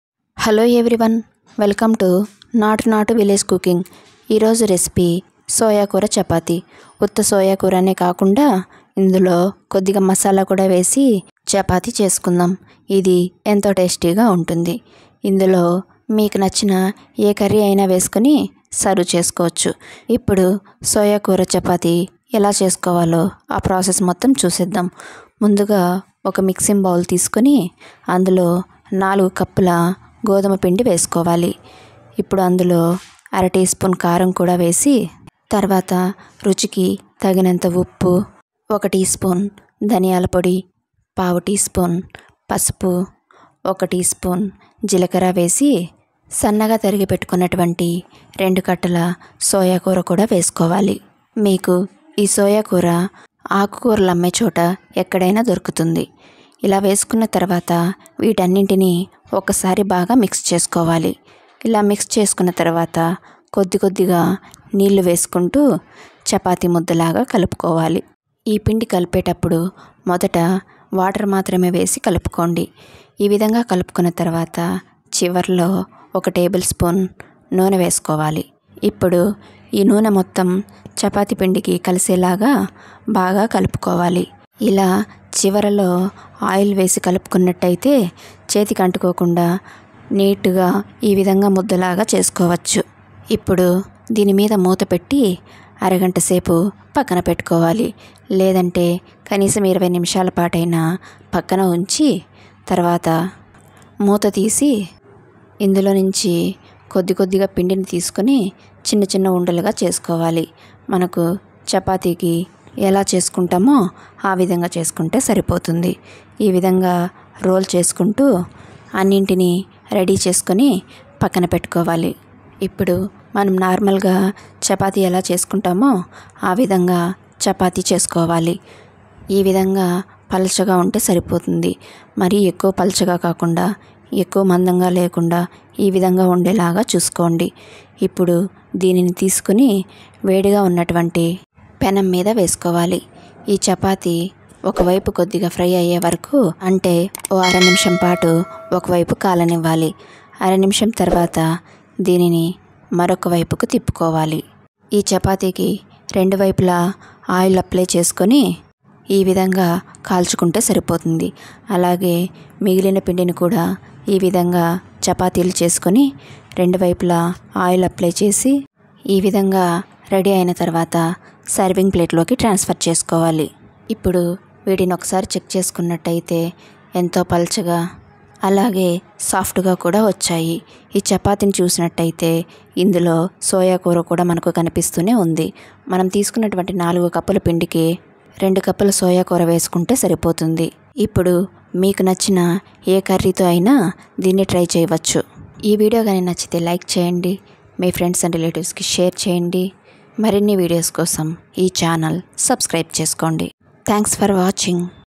సొయా కూర చపాతీ || టేస్టీగా 😋హెల్తీగా సొయా కూర చపాతీ చేసుకోవాలంటే 👉 ఇలా ట్రై చేయండి👌SOYAKURA CHAPATI
సొయా కూర చపాతీ టేస్టీగా హెల్తీగా సొయా కూర చపాతీ చేసుకోవాలంటే ఇలా ట్రై చేయండి